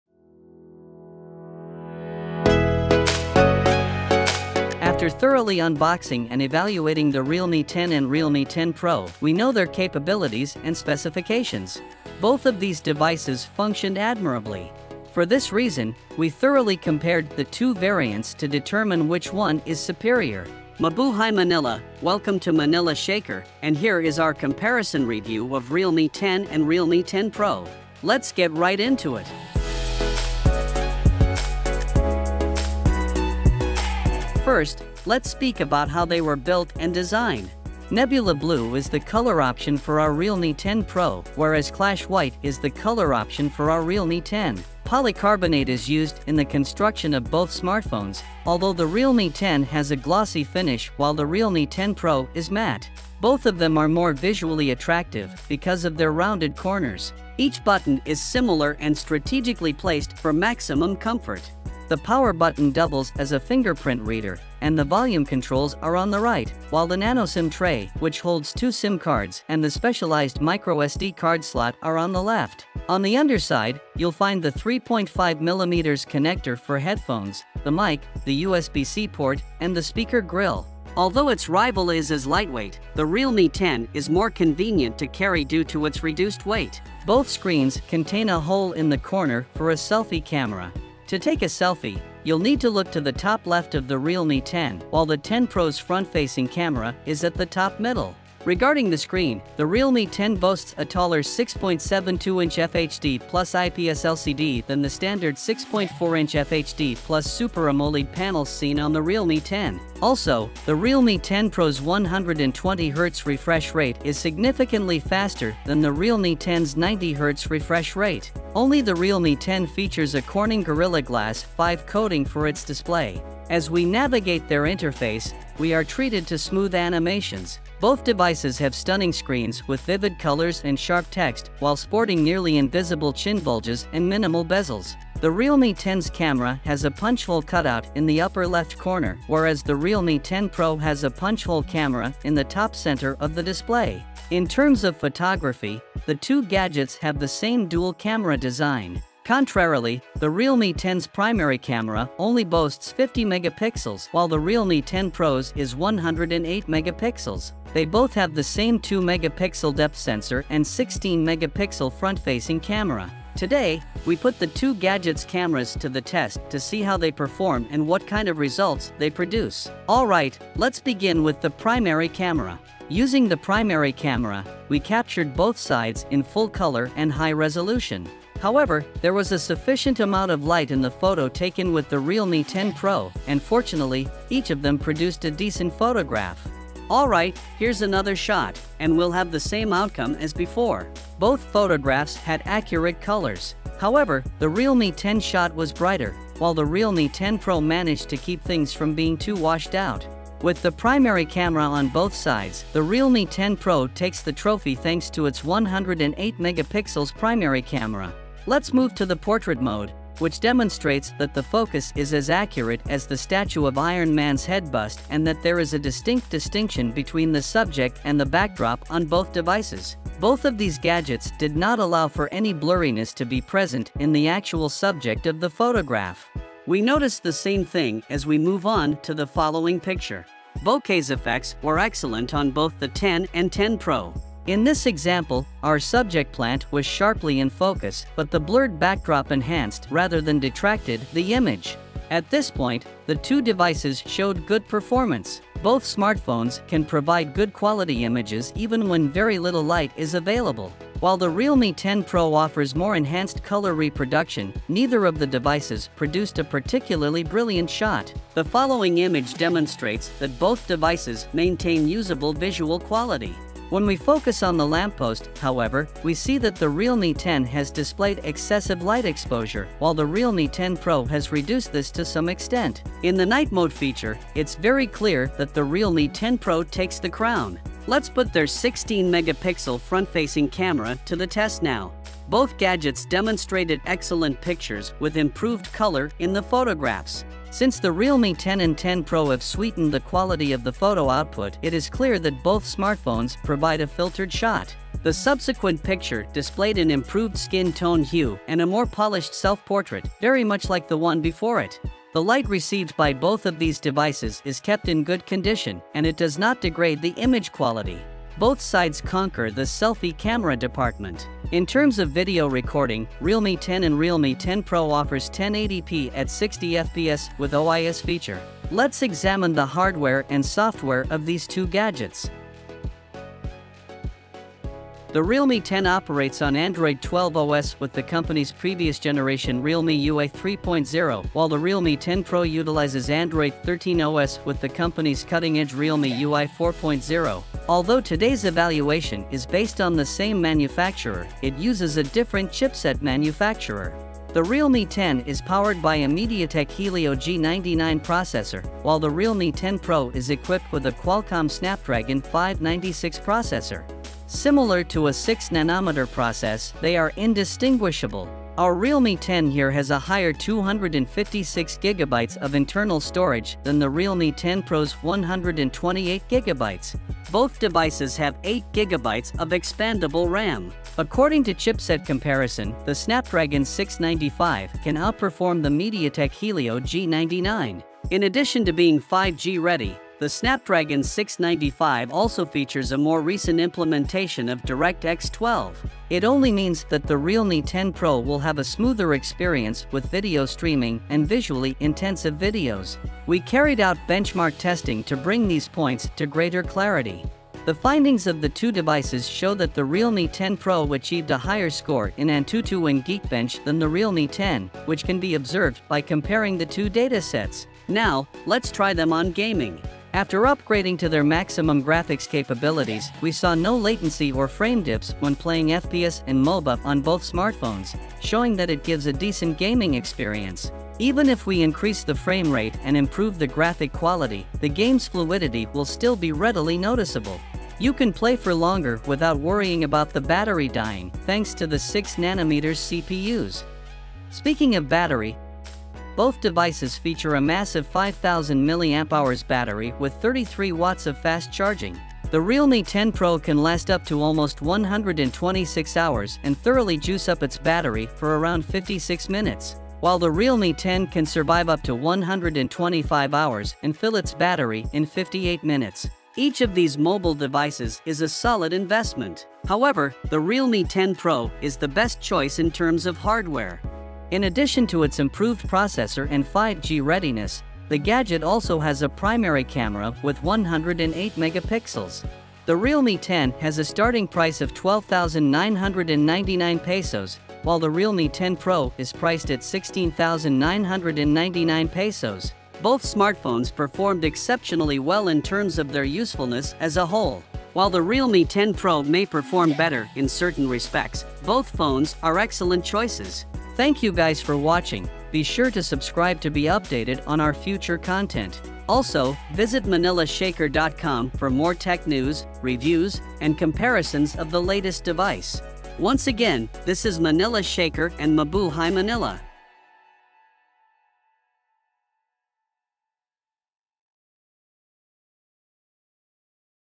Camera Clash: realme 10 vs realme 10 Pro Which One Captures the Crown?
The Ultimate Camera Showdown! Check out Manila Shaker's realme 10 and realme 10 Pro Camera Comparison and find out which of these two captures your heart!